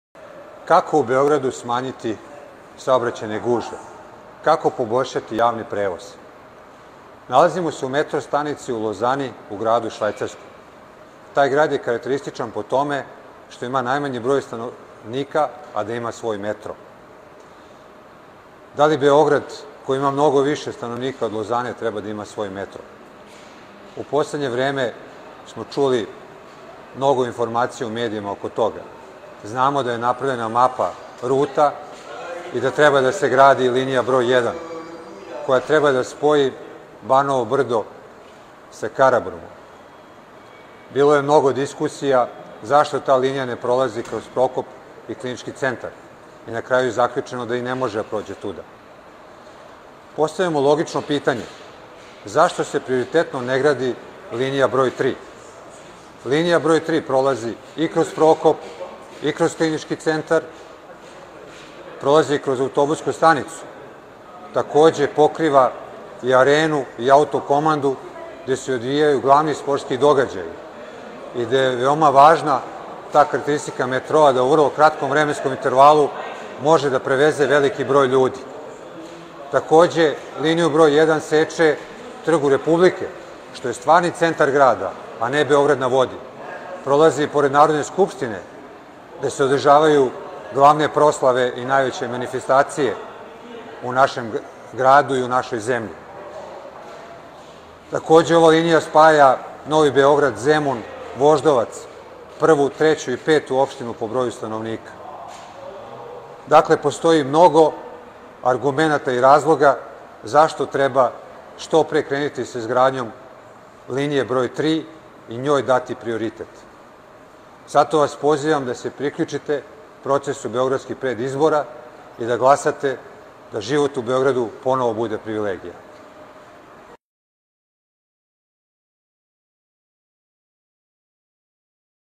Jovan Vujasinović o izgradnji metroa
Prioritet u izgradnji metroa je linija broj tri koja spaja Novi Beograd, Zemun i Voždovac, opštine sa najvećim brojem stanovnika u Beogradu.
Ruta linije broj 3 će obuhvatati lokacije:
✔️Prokop
✔️Klinički centar
✔️Autobuska stanica
✔️Arena
✔️Autokomanda
✔️Trg republike
✔️Narodna skupština
Uključite se u proces predizbora i glasajte za izgradnju metroa koji služi vašim potrebama!